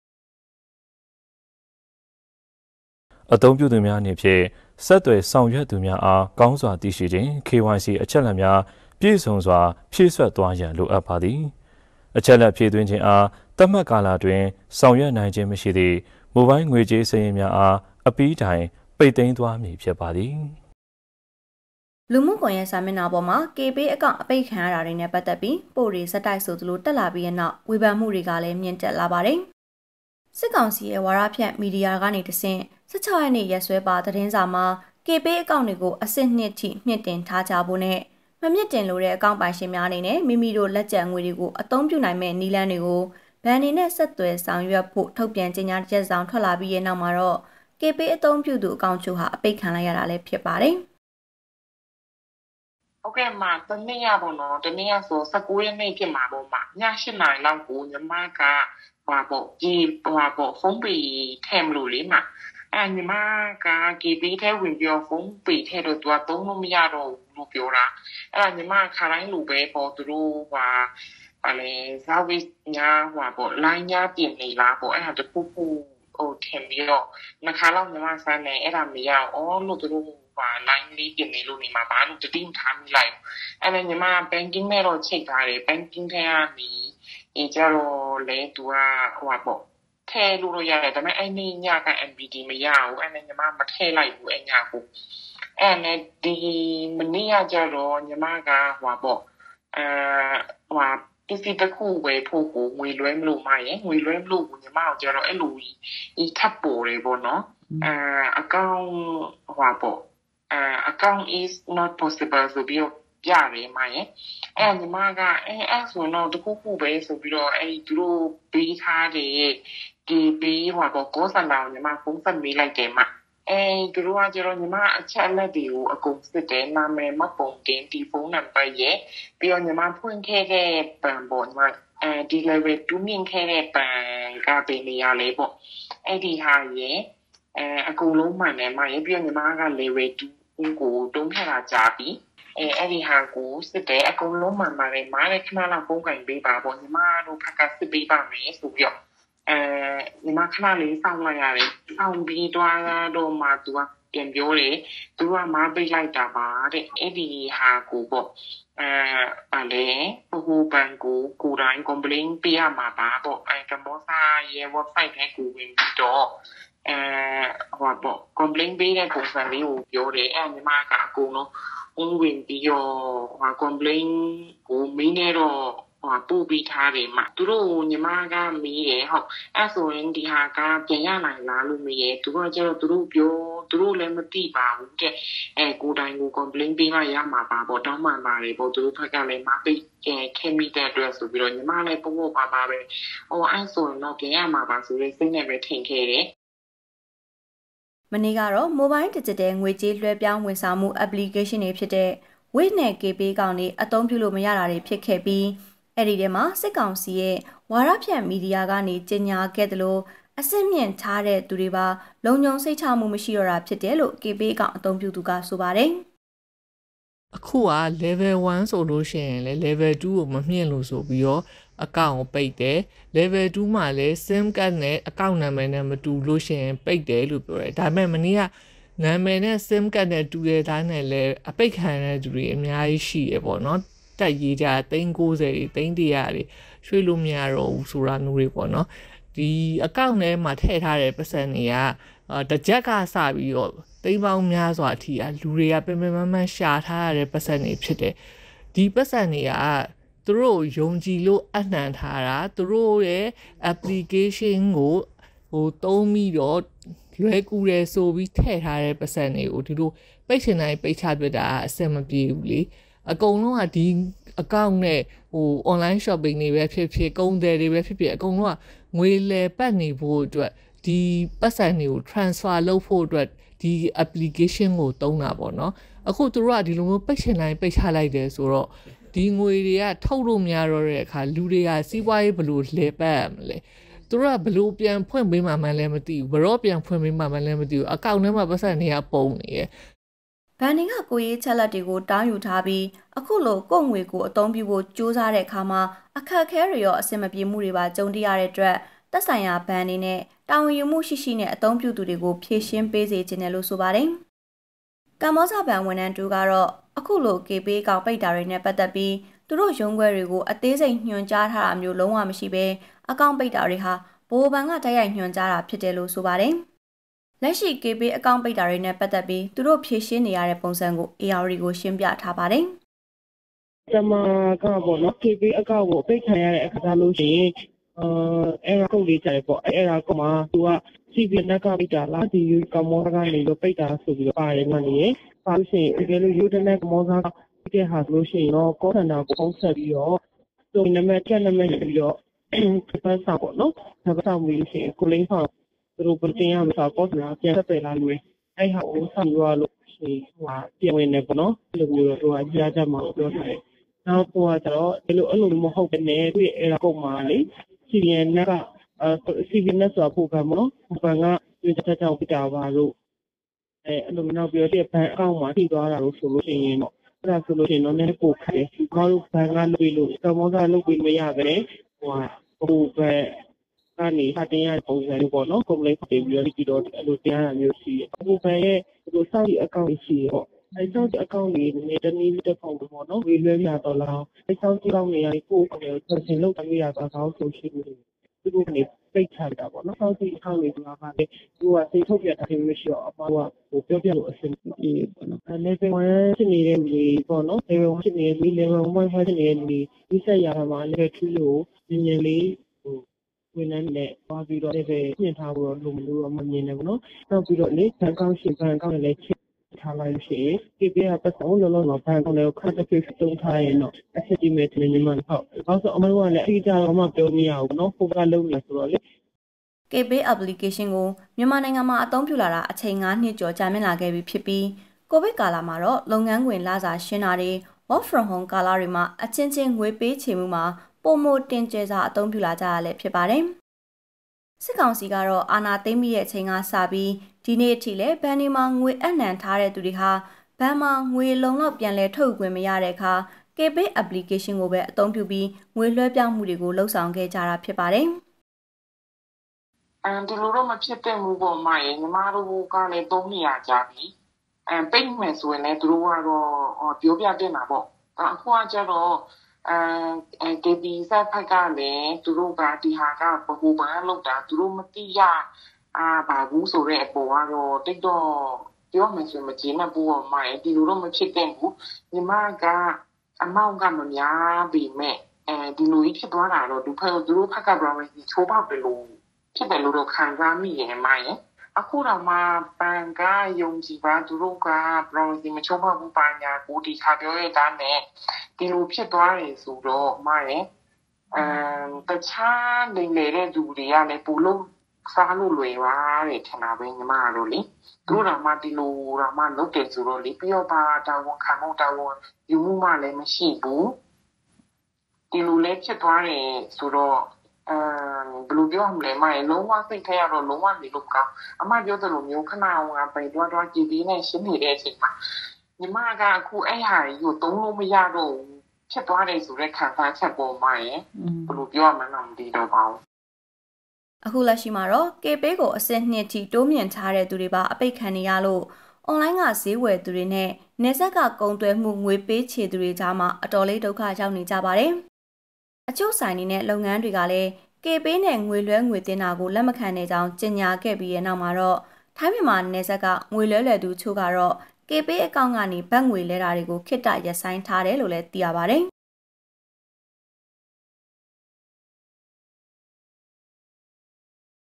အကောင့်ပိတ်တာတွေနဲ့ ပတ်သက်ပြီး တာဝန်ယူမှုရှိရှိနဲ့ ဖြေရှင်းပေးစေချင်ပါတယ်
အကောင့်ပိတ်တာတွေနဲ့ ပတ်သက်ပြီး တာဝန်ယူမှုရှိရှိနဲ့ ဖြေရှင်းပေးစေချင်ပါတယ် (ရုပ်/သံ)

ဧရာဝတီ၊ စက်တင်ဘာ ၂၃၊ ၂၀၂၂

စစ်ကောင်စီရဲ့ ဝါဒဖြန့် မီဒီယာတွေကတဆင့် ၁၆ ရက်နေ့ ရက်စွဲပါ သတင်းစာမှာ Kpay အကောင့်တွေကို အဆင့် ၂ အထိ မြှင့်တင်ထားကြဖို့နဲ့ မမြှင့်တင်လိုတဲ့ အကောင့်ပိုင်ရှင်များအနေနဲ့ မိမိတို့ လက်ကျန်ငွေတွေကို အသုံးပြုနိုင်မယ့် နည်းလမ်းတွေကို ဘဏ်တွေနဲ့ ဆက်သွယ်ဆောင်ရွက်ဖို့ ထုတ်ပြန်ကြေညာချက်တစောင် ထွက်ပေါ်လာပြီးတဲ့နောက်မှာတော့ Kpay အသုံးပြုသူတွေ အကောင့်အချို့ဟာ အပိတ်ခံလိုက်ရတာလည်း ဖြစ်ပါတယ်။

ဘဏ်တွေက ကိုယ်ရေးအချက်အလက်တွေကို တောင်းယူထားပြီး အခုလို ကိုယ့်ငွေကိုယ်အသုံးပြုဖို့ ကြိုးစားတဲ့အခါမှာ အခက်အခဲတွေရော၊ အဆင်မပြေမှုတွေပါ ကြုံတွေ့ရတဲ့အတွက် သက်ဆိုင်ရာ ဘဏ်အနေနဲ့ တာဝန်ယူမှု ရှိရှိနဲ့ အသုံးပြုသူတွေကို ဖြေရှင်းပေးစေချင်တယ်လို့ ဆိုပါတယ်။

ကမ္ဘောဇ ဘဏ်ဝန်ထမ်းတဦးကတော့ အခုလို Kpay အကောင့် ပိတ်တာတွေနဲ့ ပတ်သက်ပြီး သူတို့ ရုံးခွဲတွေကို အသေးစိတ်ညွှန်ကြားထားတာမျိုး လုံးဝမရှိပဲ၊ အကောင့် ပိတ်တာတွေဟာ ဗဟိုဘဏ်က တိုက်ရိုက် ညွှန်ကြားတာဖြစ်တယ်လို့ ဆိုပါတယ်။